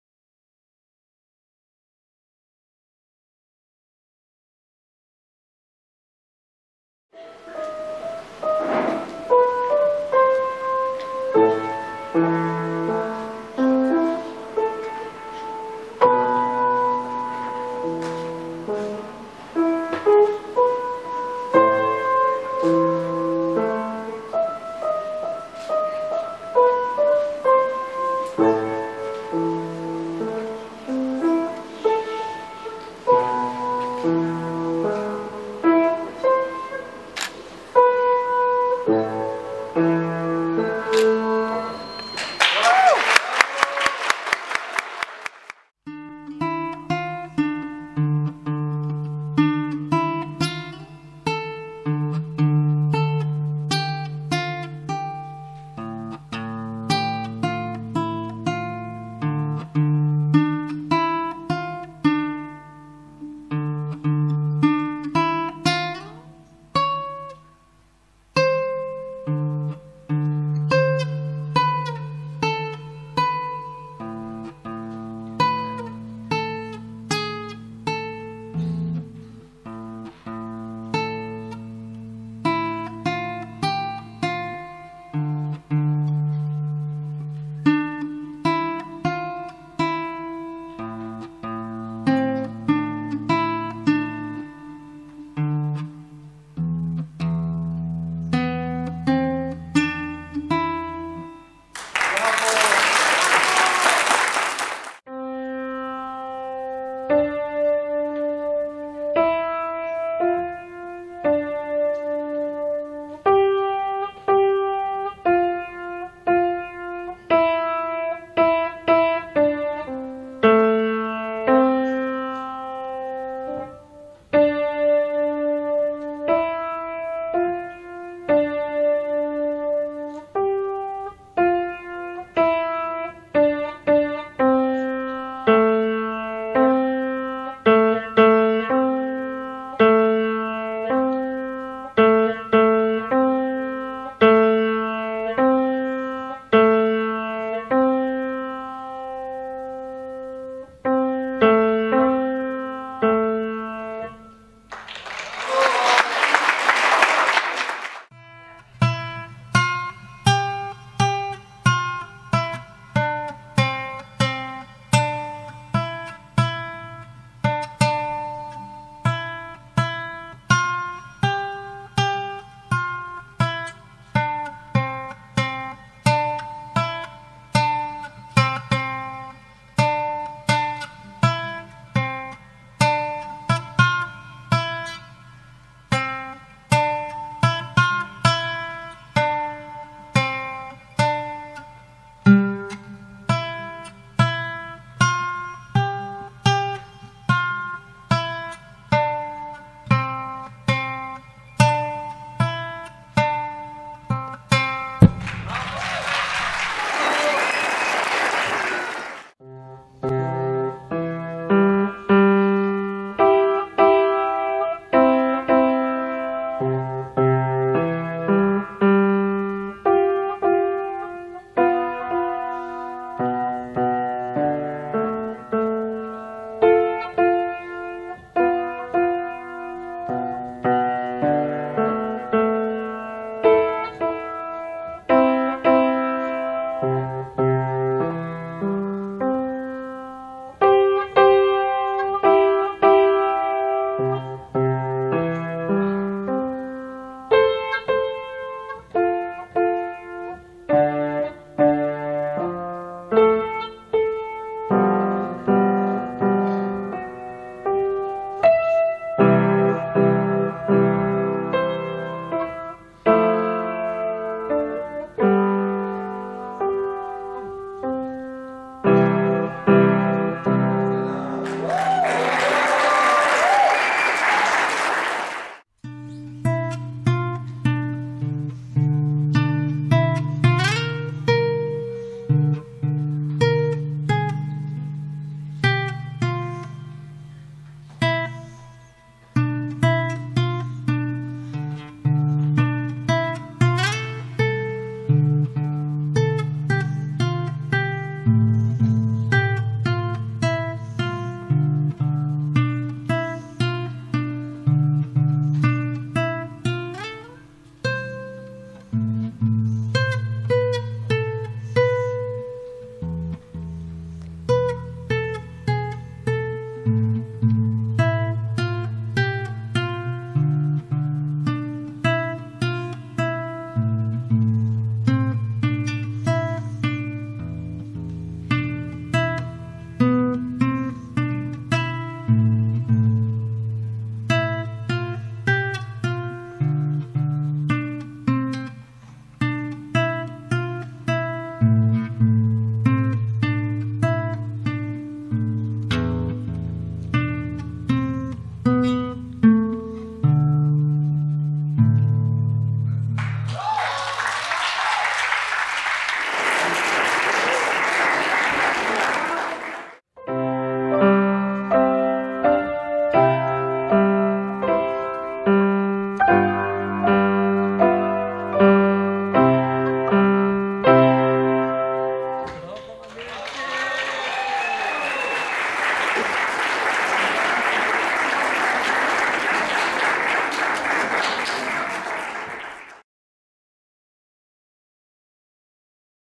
I am special, I am unique, I am me… Albashaer Concert (1)
أداء:
00:00 مقدمة
00:06  1- ماثيو حايك / بيانو
00:43  2- كارولينا توما / غيتار
01:40  3- نايا اليازجي / بيانو
02:31  4- كيفن حبيس / غيتار
03:20  5- جويا شلش / بيانو
04:24  6- جاد سعد الدين / غيتار
05:45  7- لارا الشهابي + طارق الشهابي / بيانو

مونتاج: غالب زيدان
تصوير: غالب زيدان - يسرى الكوري

مدرسة البشائر الوطنية الخاصة
سوريا - ريف دمشق - قرى الأسد